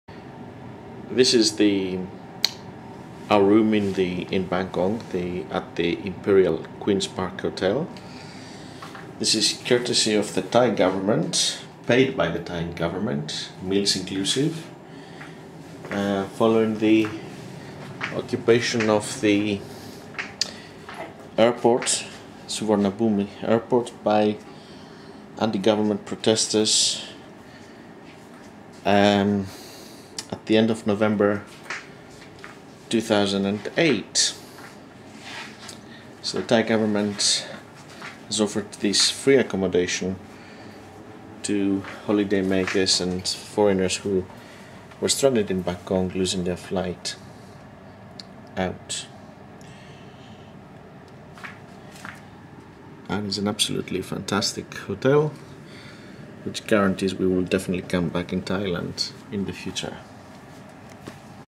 Imperial Queen Park Hotel standard room
Imperial Queen Park's Hotel standard room. We were lucky to stay there, all inclusive, courtesy of the Thai People after we lost our flight as a result of the occupation of Bangkok's airports by protesters during late November/early December 2008